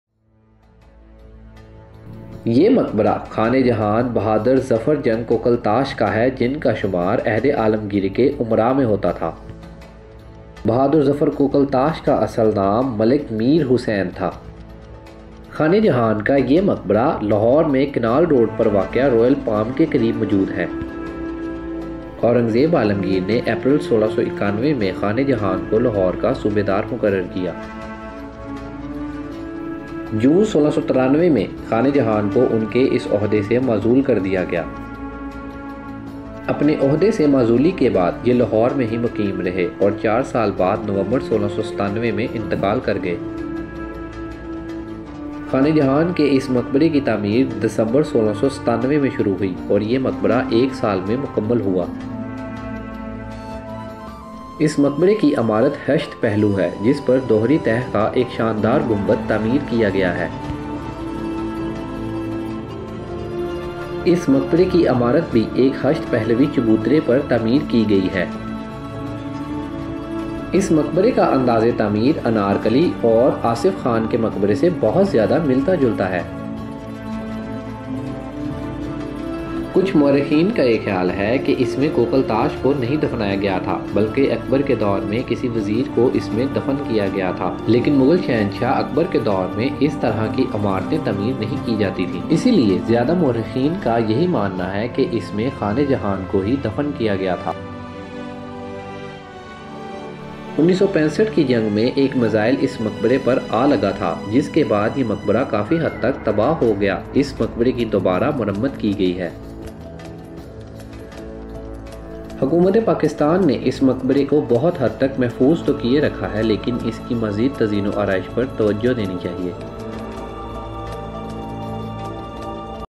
Tomb Of khan-E-Jahan Bahadur zafar jang Kokaltash ll مقبرہ خانِ جہاں بہادر ظفر کوکلتاش ll Since 1697
This is the tomb of Khan e jahan Bahadur Zafar Jang Kokaltain who was a nobleman of the court of Aurangzeb Alamgir. He was appointed as the governor of Lahone province in April 1691 AD and removed from his position in June 1693 AD. After his removal he stayed in Lahore and died four years later, in November 1697 CE
The construction of his tomb was started in December 1697 AD and completed in a year. The tomb structure is octagonal and surmounted by an imposing double-shell dome. The tomb is resting on an octagonal platform. Although its construction resembles the tombs of Anarkalli and Nusrat Khan, its fair-faced brick embellishment pronounces its individuality.

یہ مقبرہ خانِ جہاں ظفر جنگ کلتاش کا ہے جن کا شمار عہدِ جہانگیری کے امرا میں ہوتا تھا۔ اورنگزیب نے انہیں لاھور کا صوبیدار مقرر کیا اور جون 1693ء میں انہیں ان کے عہدے سے معزول کر دیا۔ معزولی کے بعد سے لاہور میں ہی مقیم رہے اور عرصہ چار برس بعد نومبر 1697  میں انتقال کر گئے۔

ان کے مقبرے کی تعمیر دسمبر 1697ء میں شروع ہوئی اور ایک سال میں پایہ تکمیل کو پہنچی۔ مقبرے کی عمارت ہشت پہلو ہے جس پر دوہری تہہ کا ایک شاندار گنبد تعمیر کیا گیا ہے۔ مقبرے کی عمارت ایک ہشت پہلوی چبوترے پر تعمیر کی گئی ہے۔ گو کہ اس مقبرے کا انداز تعمیر مقبره انار کی اور نصرت خان سے مشابہ ہے لیکن اس کی خشتی تعمیر اسے انفرادیت بخشتی ہے۔

Video graphy By :  Abbad Arif
Video Edit By      : Abbad Arif
Voice over By     : Abbad Arif